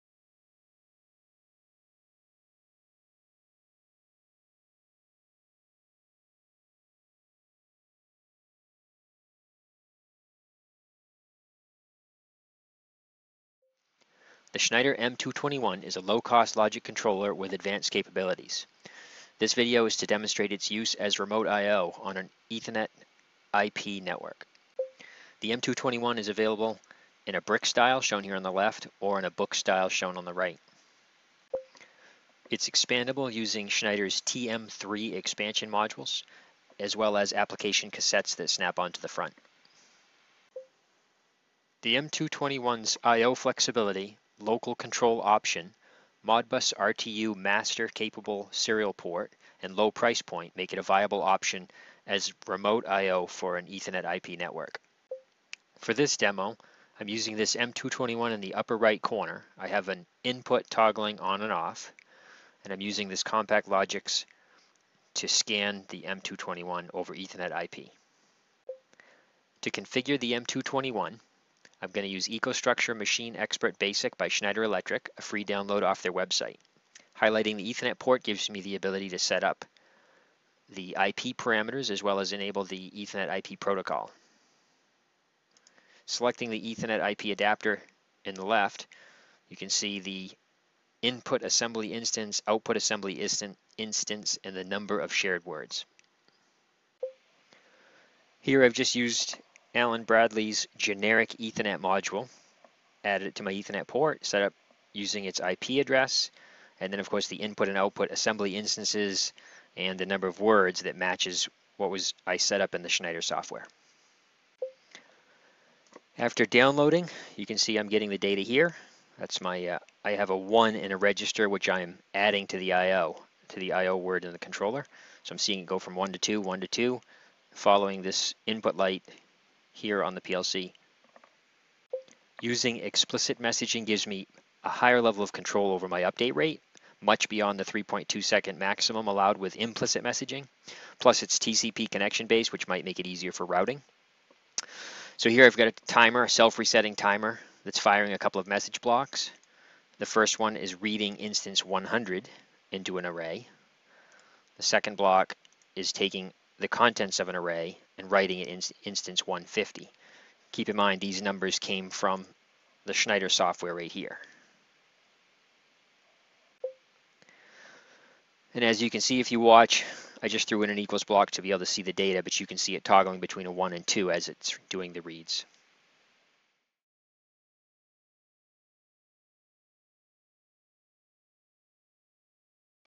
The Schneider Modicon M221
IAS Applications Engineer Donnie Dauphin goes through the paces of utilizing the Schneider Modicon M221 logic controller as remote I/O on an Ethernet IP network. and to learn more about this and a wide array of automation equipment and services, visit Industrial Automation Supply